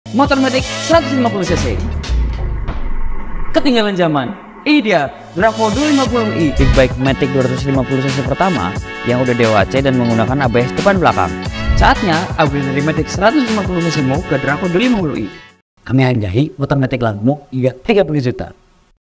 UPGRADE MOTOR MATIC 150 CC MU KE DRACO 250i
DRACO 250i, Satu satunya BIG BIKE Matic 250 cc yang sudah DOHC.
motor dari brand yang sudah banyak di pakai di Eropa sampai 80 negara.

Draco 250i ini, Authorized dealernya hanya di The Biker Shop!